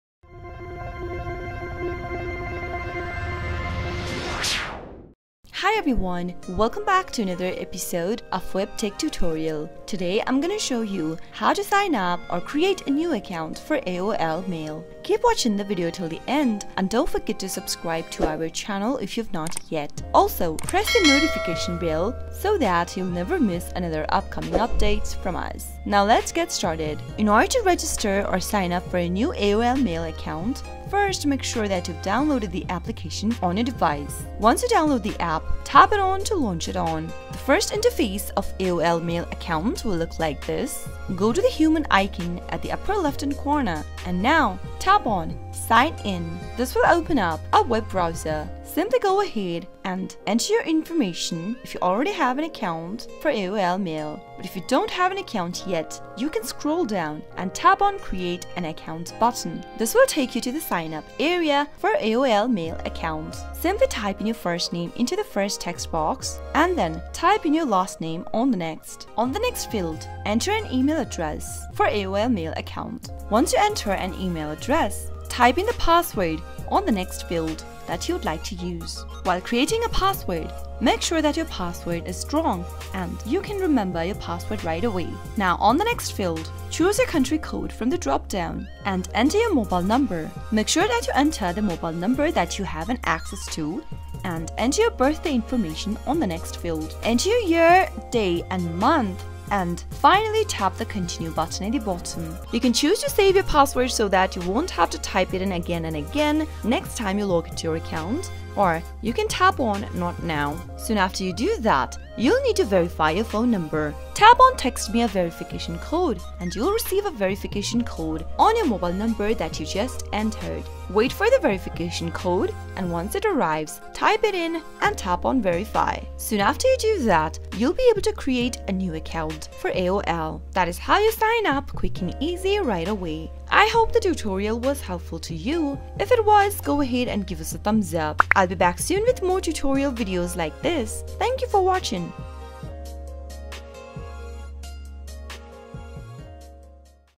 AOL Mail Sign Up: How to Create/Open AOL Mail Account 2022? mail.aol.com
Here we present you latest and updated ways of signing up for AOL mail account easily in 2 minutes. In the video, you will see how to easily open a brand new account on AOL mail and start using it right away.

AOL Mail Sign Up Process Explained in Details.